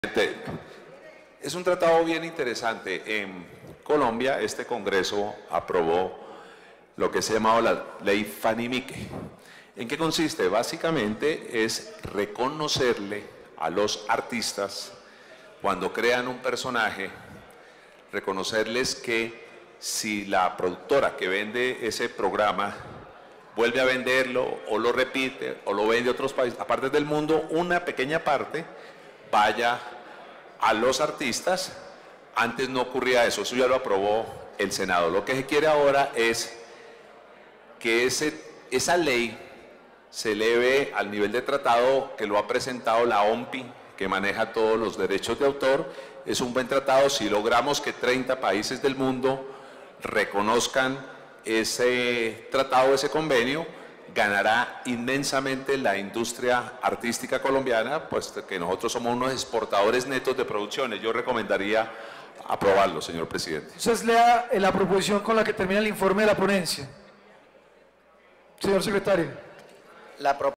Senador Velasco presenta ponencia sobre el Tratado de Beijing
Ponencia en la Plenaria del jueves 17 de noviembre del 2016 sobre el tratado de Beijing. lea más